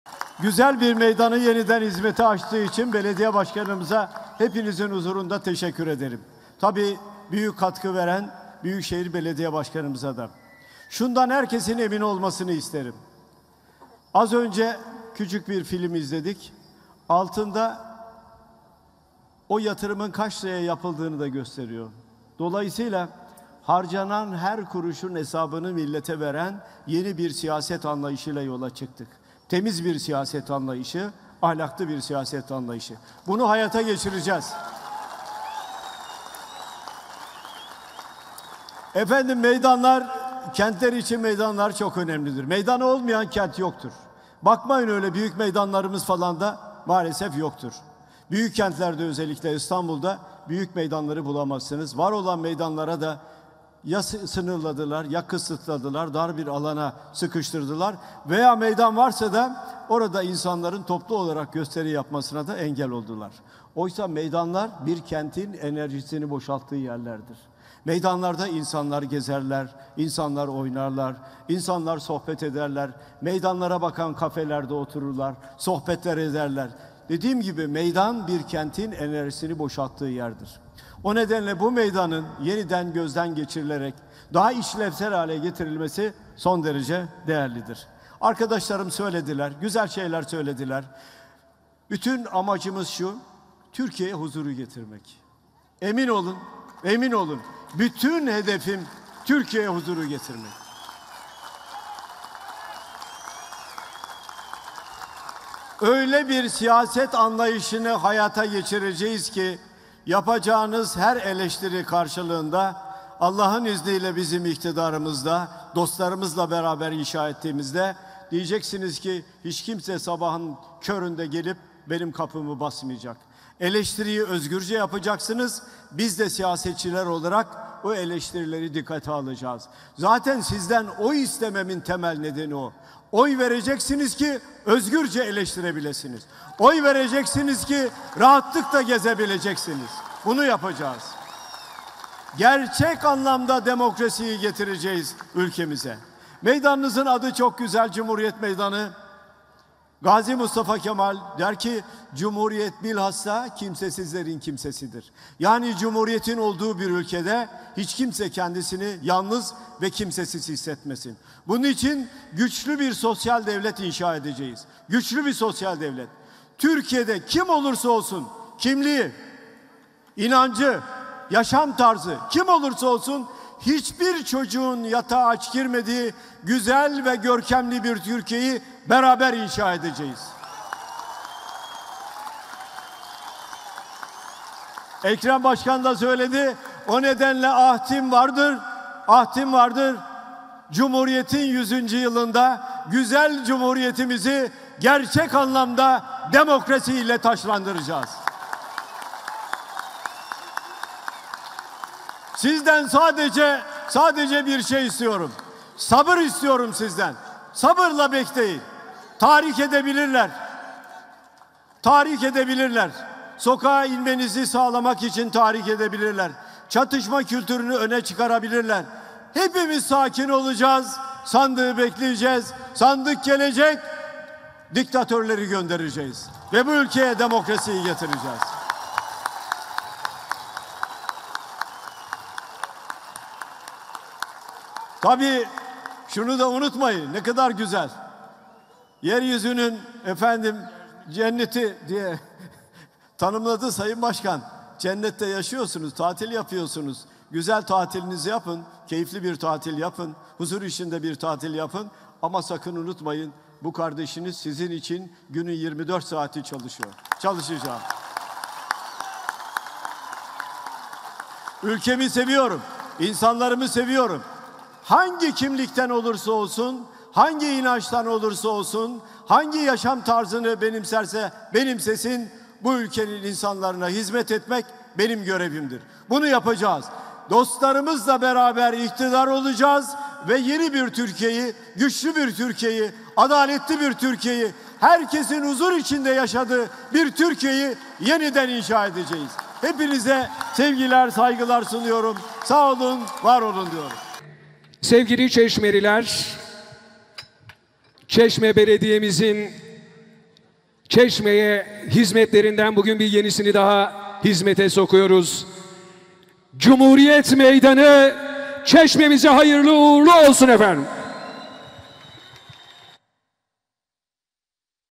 KEMAL KILIÇDAROĞLU - ÇEŞME CUMHURİYET MEYDANI TOPLU AÇILIŞ TÖRENİ 11/09/2021
Cumhuriyet Halk Partisi Genel Başkanı Kemal Kılıçdaroğlu, Çeşme'de Cumhuriyet Meydanı Açılış Töreninde konuştu.